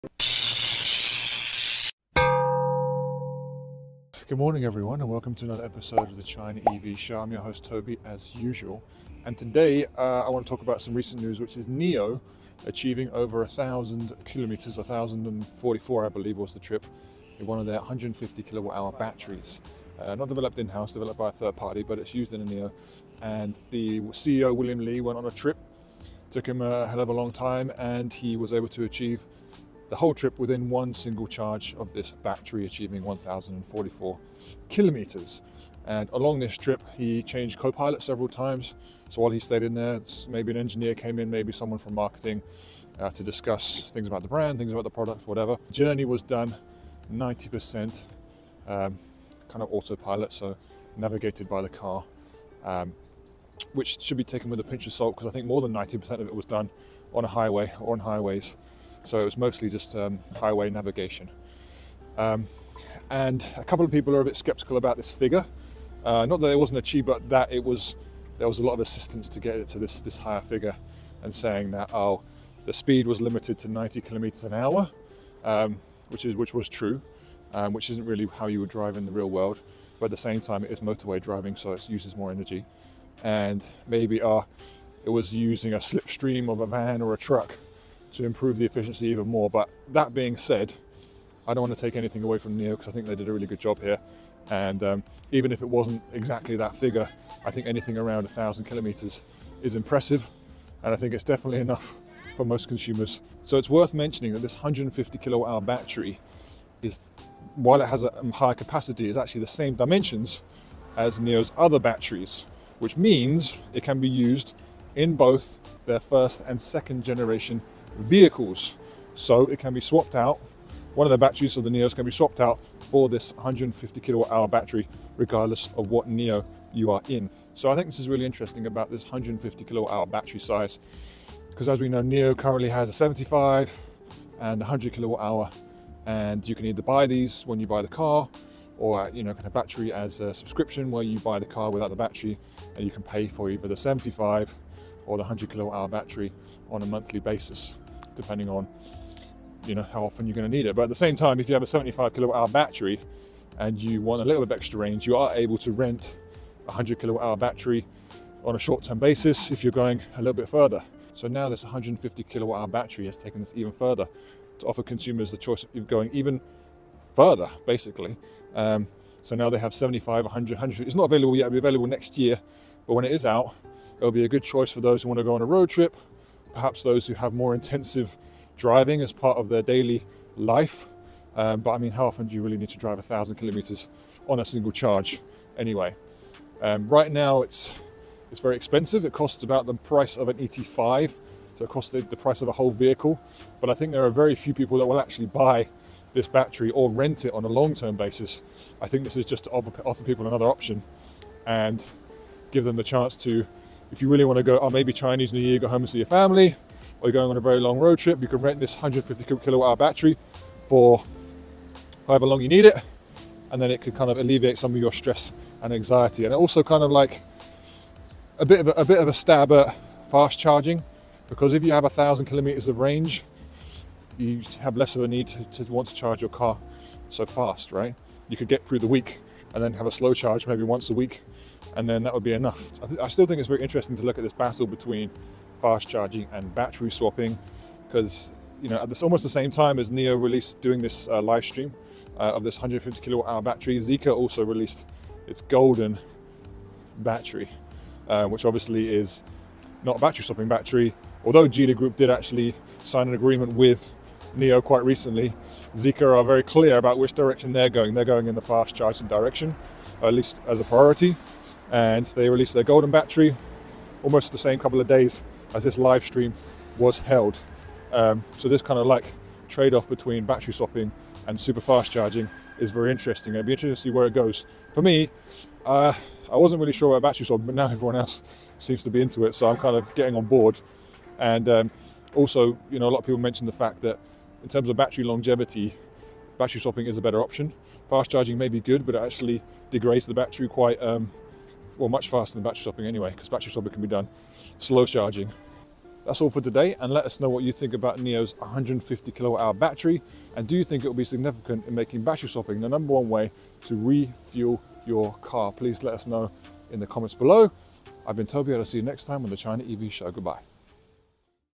What NIO's 1000+km Battery Means for the EV Industry
Join us in exploring the groundbreaking strides of NIO's 150kWh battery and their autonomous driving feat! Throughout the trip, William Li switched co-pilots while the NIO car autonomously navigated over 90% of the journey, maintaining a capped speed of 90kmh, primarily on motorways.

We delve into the details: the skeptics' concerns, like slipstreaming for efficiency, and the implications of this milestone, considering it's under conditions similar to other tests. The 150kWh battery, with its increased energy density and compatibility with NIO's range, holds vast potential. However, its current cost and concept stage pose challenges for widespread adoption.

The battle between battery swapping and fast charging heats up, raising questions about longevity and efficiency. Share your thoughts on this game-changing battery and the evolving landscape of electric mobility in the comments!